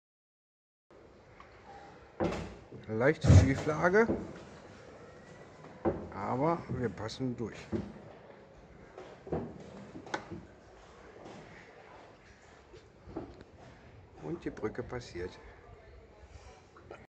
Gondelfahrt in Venedig unter eine Brücke durch
Tja, wer bei Hochwasser fährt, kann etwas besonderes in der Gondel erleben. Damit Die Gondel durch die Brücken passt, bekommt sie etwas Schieflage...